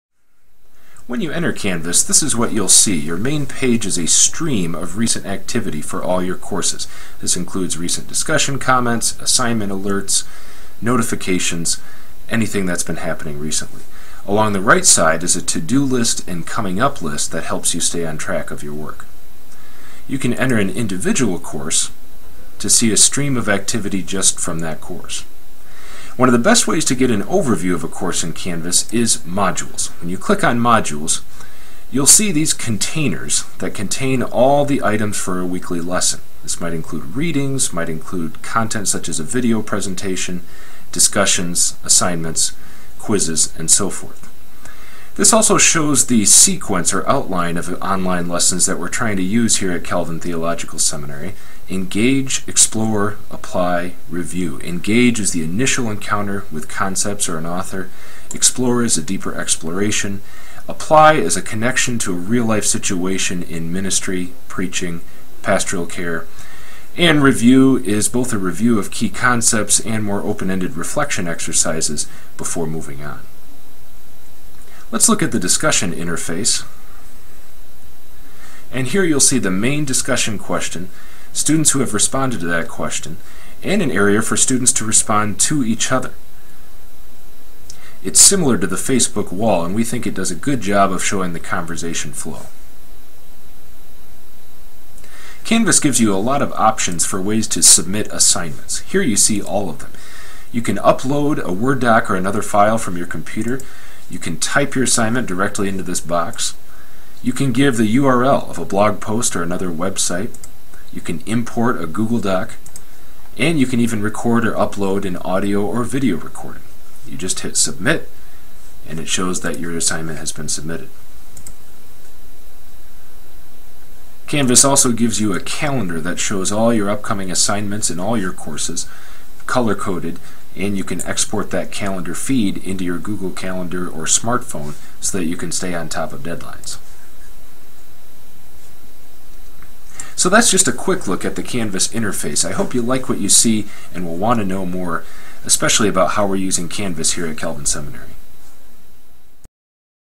Tour of Canvas for online learning at Calvin Seminary - Fall 2012
Overview of our new course management system, Canvas, by Nathan Bierma, Educational Technologist at Calvin Seminary.

To apply for an online course or degree program at Calvin Seminary, go